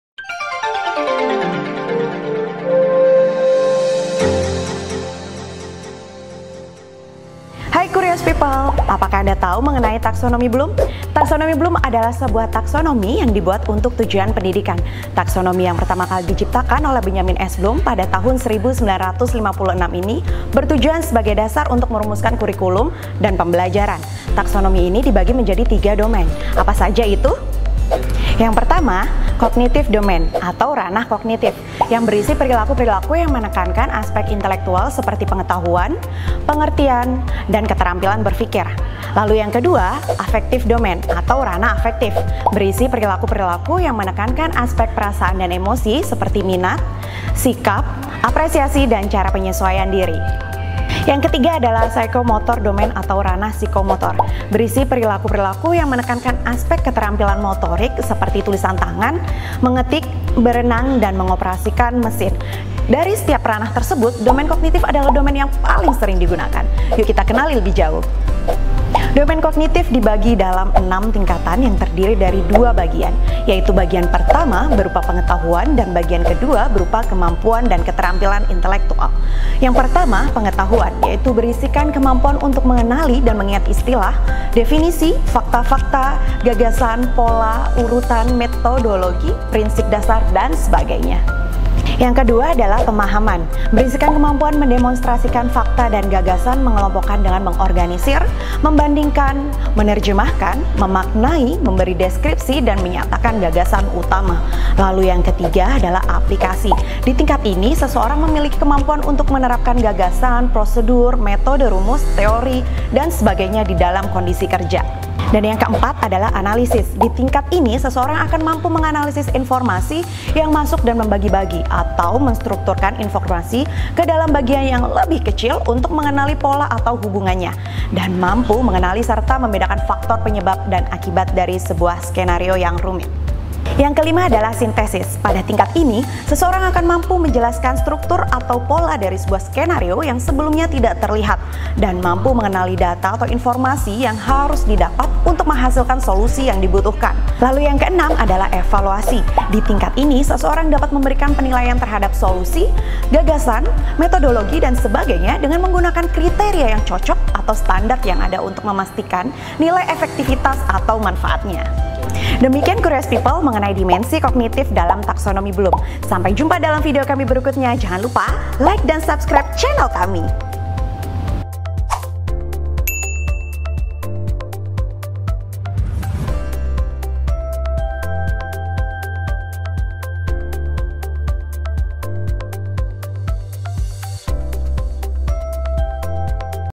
Taksonomi Bloom ( Bloom Taxonomy )
Halo Curious People!
Tahukah kamu apa itu Taksonomi Bloom?

Credit :
- Talent : Anzil Firdausi Nuzula

Simak yuk videonya!
Supaya dapat insight baru hari ini loh guys..

Follow IG kami :
Banyak konten yang menarik loh..

Taksonomi ini dibagi menjadi tiga domain, apa saja itu? yaitu:
1. Cognitive Domain (Ranah Kognitif), yang berisi perilaku-perilaku yang menekankan aspek intelektual, seperti pengetahuan, pengertian, dan keterampilan berpikir.

2. Affective Domain (Ranah Afektif) berisi perilaku-perilaku yang menekankan aspek perasaan dan emosi, seperti minat, sikap, apresiasi, dan cara penyesuaian diri.

3. Psychomotor Domain (Ranah Psikomotor) berisi perilaku-perilaku yang menekankan aspek keterampilan motorik seperti tulisan tangan, mengetik, berenang, dan mengoperasikan mesin.

Dari setiap ranah tersebut, domain kognitif adalah domain yang paling sering digunakan. Yuk kita kenali lebih jauh.

Domain kognitif dibagi dalam 6 tingkatan yang terdiri dari dua bagian, yaitu Bagian pertama berupa Pengetahuan dan bagian kedua berupa Kemampuan dan Keterampilan Intelektual.
1. Pengetahuan
Berisikan kemampuan untuk mengenali dan mengingat istilah, definisi, fakta-fakta, gagasan, pola, urutan, metodologi, prinsip dasar, dsb.

2. Pemahaman
Berisikan kemampuan mendemonstrasikan fakta dan gagasan mengelompokkan dengan mengorganisir, membandingkan, menerjemahkan, memaknai, memberi deskripsi, dan menyatakan gagasan utama
3. Aplikasi
Di tingkat ini, seseorang memiliki kemampuan untuk menerapkan gagasan, prosedur, metode, rumus, teori, dsb di dalam kondisi kerja.

4. Analisis
Di tingkat ini, seseorang akan mampu menganalisis informasi yang masuk dan membagi-bagi atau menstrukturkan informasi ke dalam bagian yang lebih kecil untuk mengenali pola atau hubungannya, dan mampu mengenali serta membedakan faktor penyebab dan akibat dari sebuah skenario yg rumit.
5. Sintesis (Synthesis)
Pada tingkat ini, seorang akan mampu menjelaskan struktur atau pola dari sebuah skenario yang sebelumnya tidak terlihat, dan mampu mengenali data atau informasi yang harus didapat untuk menghasilkan solusi yg dibutuhkan.
6. Evaluasi (Evaluation)
Di tingkat ini, seseorang dapat memberikan penilaian terhadap solusi, gagasan, metodologi, dsb dengan menggunakan kriteria yang cocok atau standar yg ada untuk memastikan nilai efektivitas atau manfaatnya.